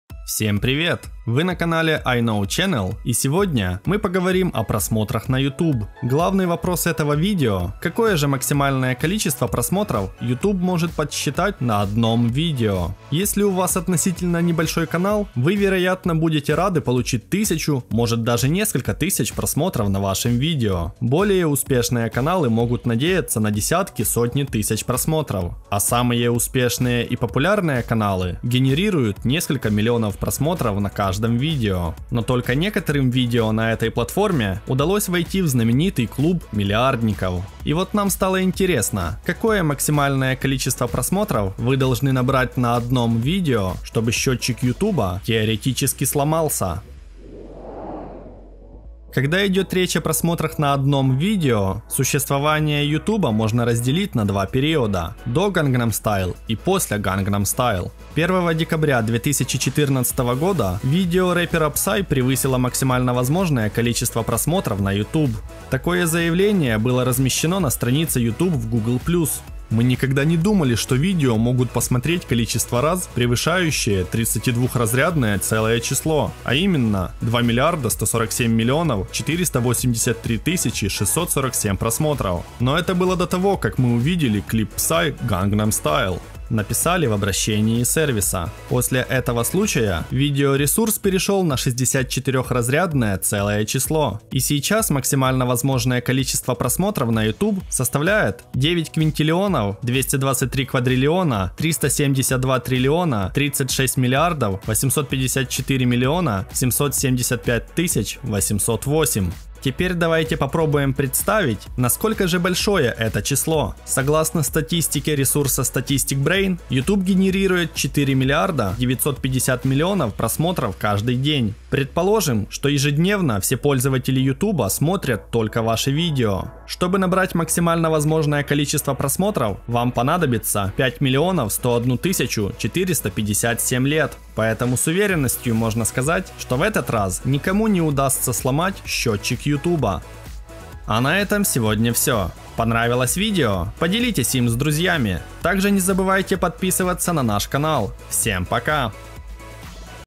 Какое максимальное количество просмотров на YouTube?
Какое же максимальное количество просмотров YouTube может подсчитать на одном видео? Смотрите ответ в этом выпуске.